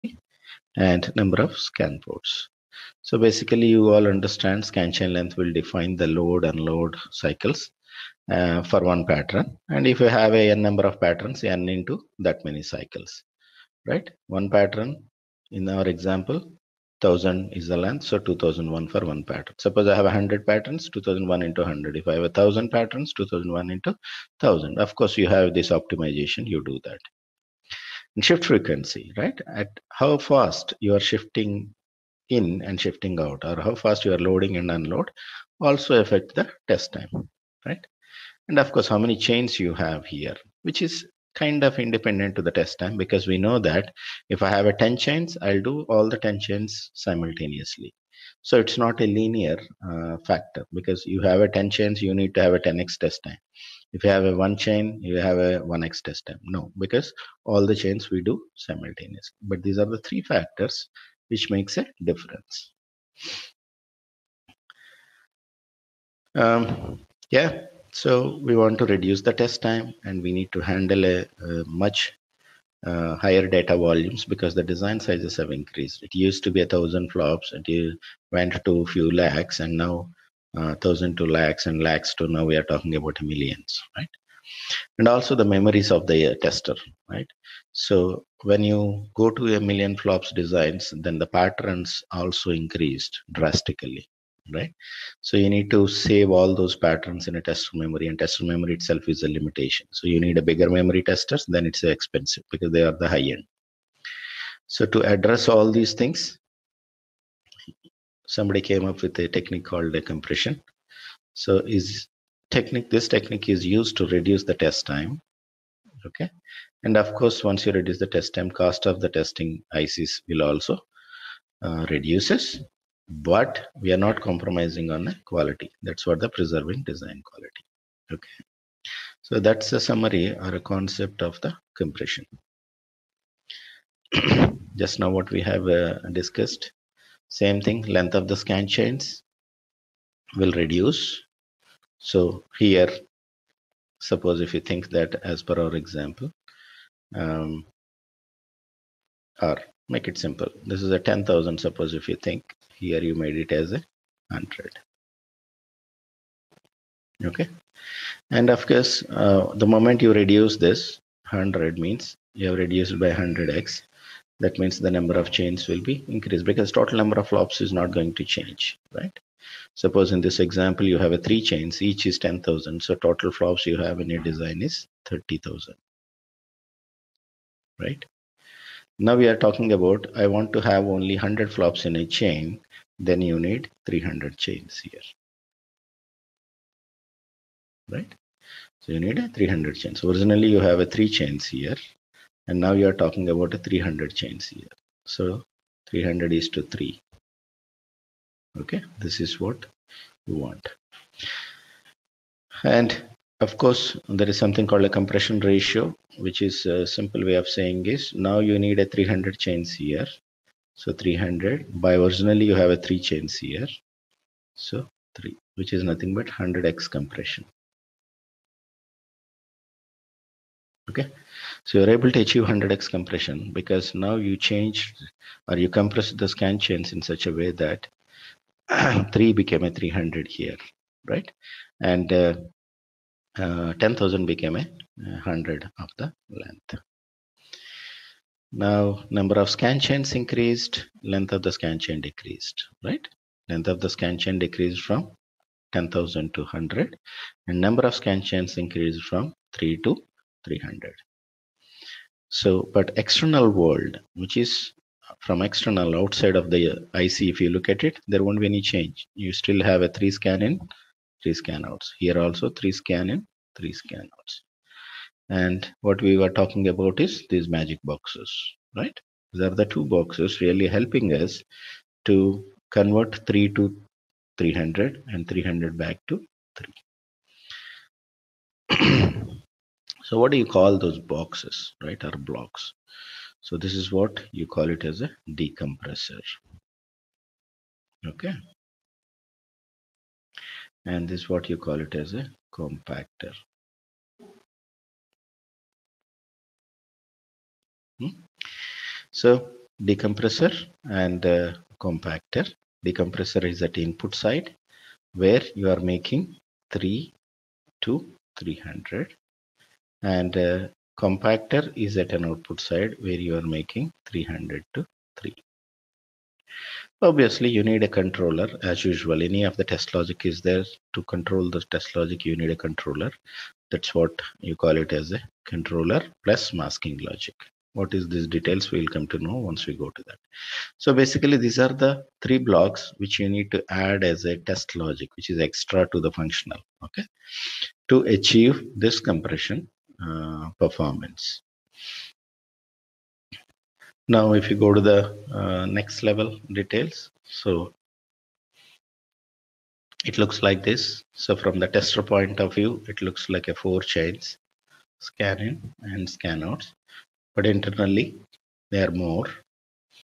DFT Online Course Demo - Lecture Session
This is small video snippet from the recent DFT online class, to give an idea on how online class is delivered .